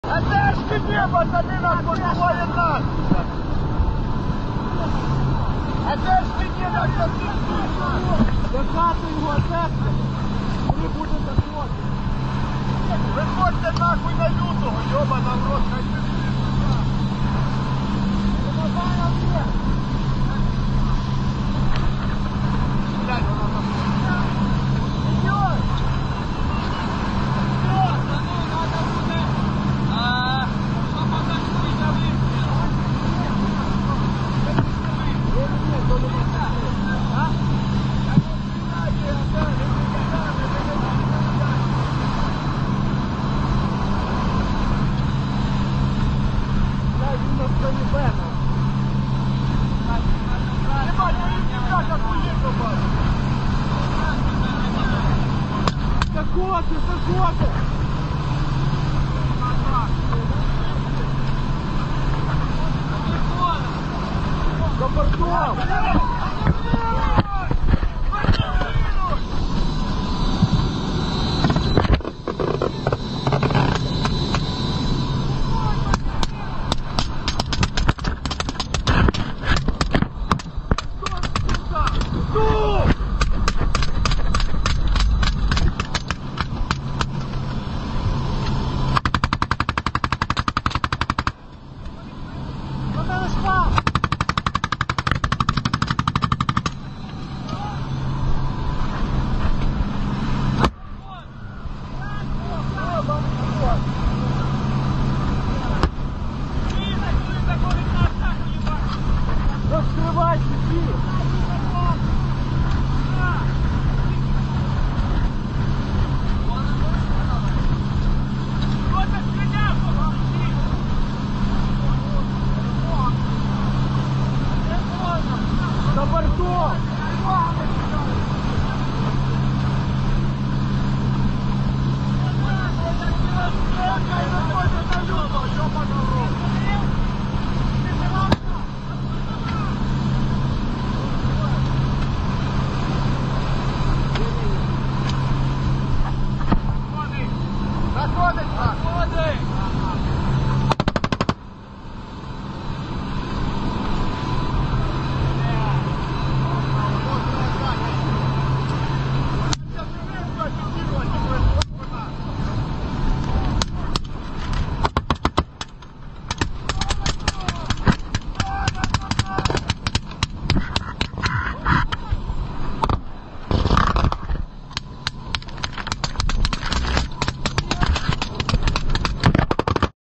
Перший бій в Криму!
Підрозділ ГУР МО Артан виконує бойове завдання на території Українського Криму.

Найактуальніші новини України та світу з різних джерел.

Ви можете підтримати цей канал:
💲 - Приват 💳: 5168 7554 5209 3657
💰- BTC 1BH1VhTNzdsNh4jJgVtbPoxrrvt4Bn5yQi